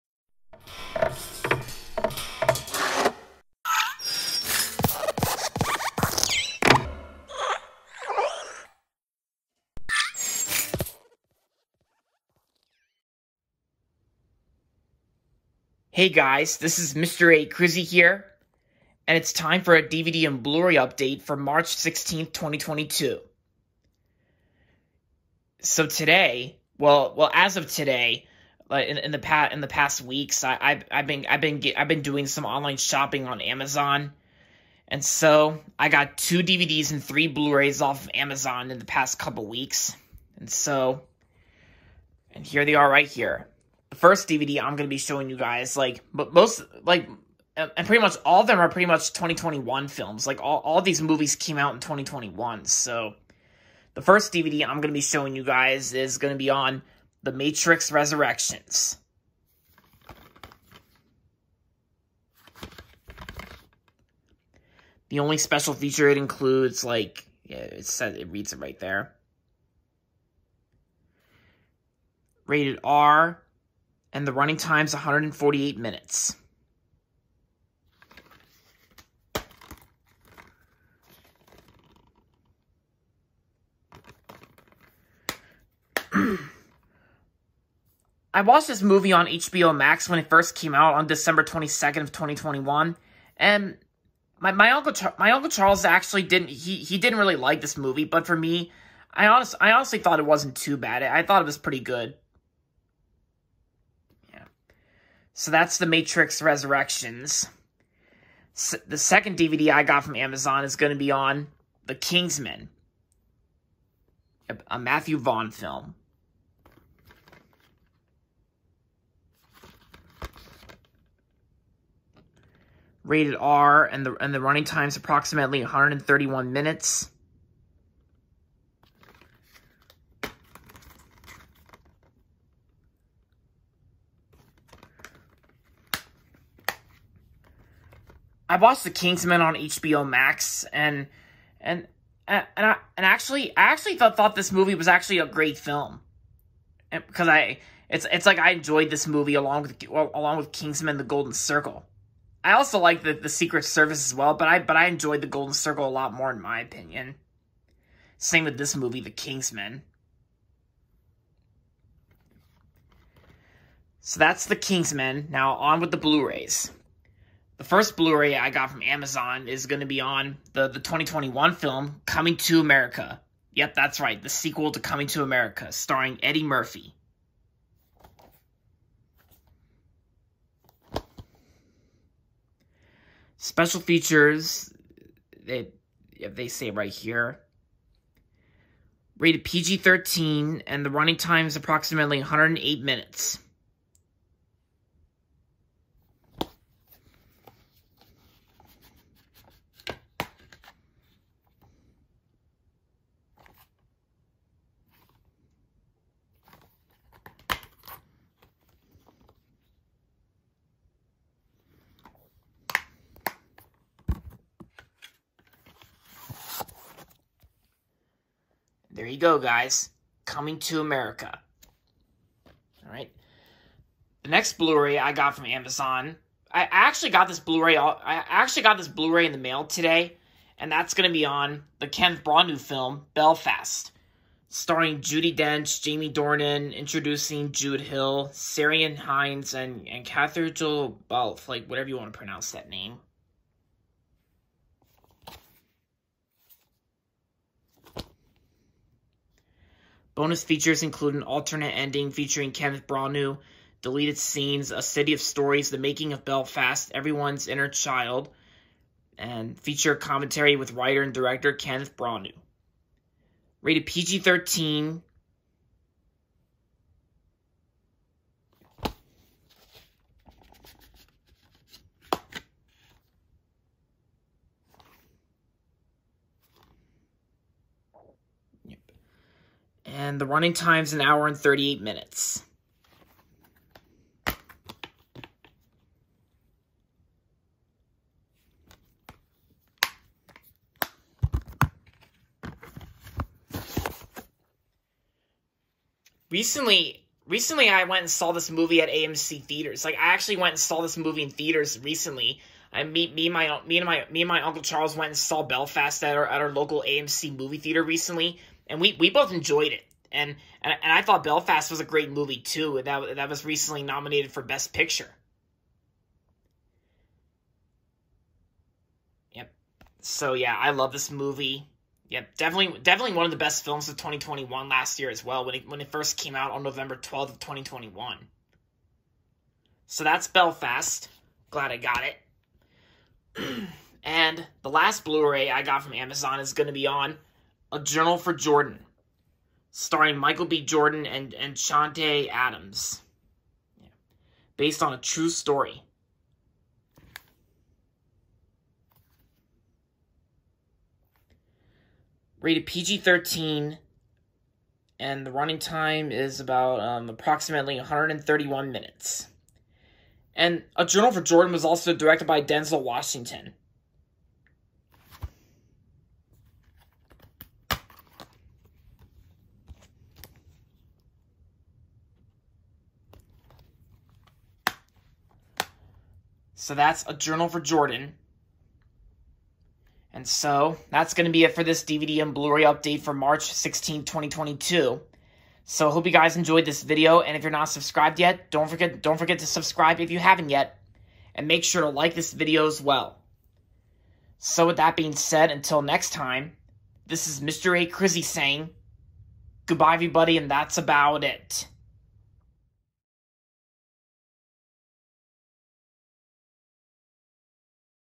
DVD/Blu-ray Update for March 16th, 2022
I got two DVDs and three Blu-rays off of Amazon.

DVDs:
The Matrix Resurrections (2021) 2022 DVD
The King's Man (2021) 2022 DVD

Blu-rays:
Coming 2 America (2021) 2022 Blu-ray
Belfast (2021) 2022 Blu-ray
A Journal for Jordan (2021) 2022 Blu-ray